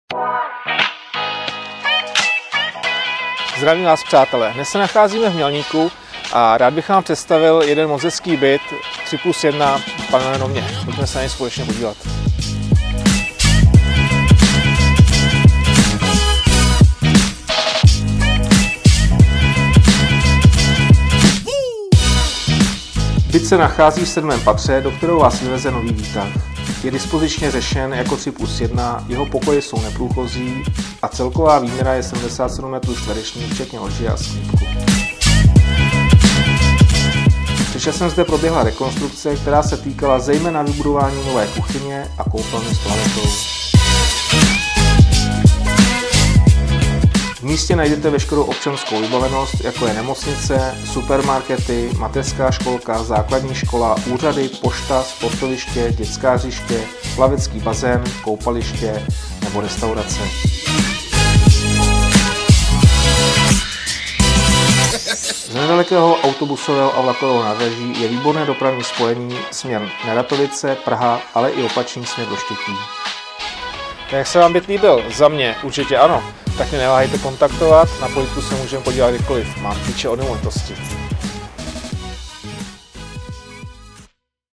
Byt 3+1 Mělník
Prostorný byt o velikosti 3+1 na prodej.  Celková výměra je 77 m2, nachází se v sedmém patře zrekonstruovaného panelového domu v ulici Bezručova v Mělníku. Jednotka je v pečlivě udržovaném stavu z velké části po rekonstrukci – nové rozvody, omítky, SDK podhledy, koupelna, kuchyně, plastová okna, PVC podlaha s dekorem dřeva.